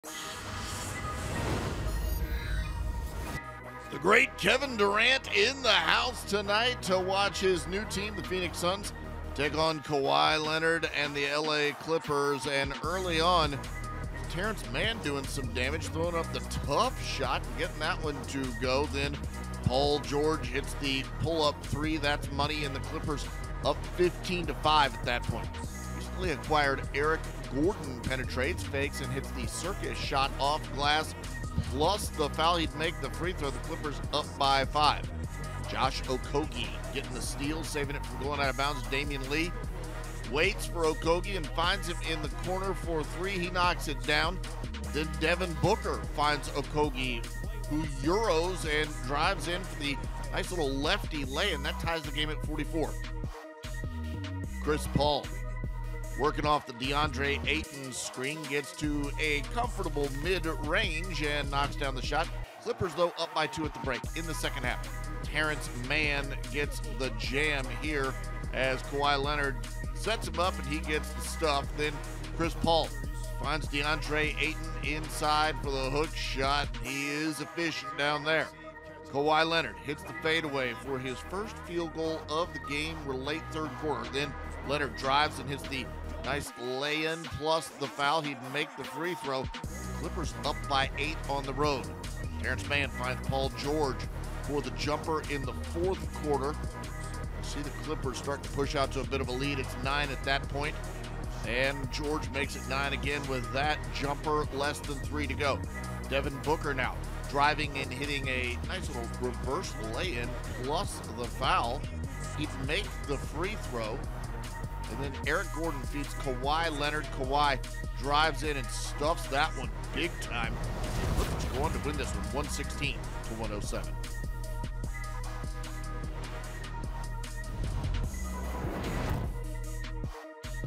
Game Recap: Clippers 116, Suns 107
The Clippers defeated the Suns, 116-107. Paul George recorded 26 points, six rebounds and five assists for the Clippers, while Terance Mann added 26 points, four rebounds and three assists in the victory. Josh Okogie tallied 24 points with a career-high six 3PM for the Suns in the losing effort. The Clippers improve to 33-28, while the Suns fall to 32-28.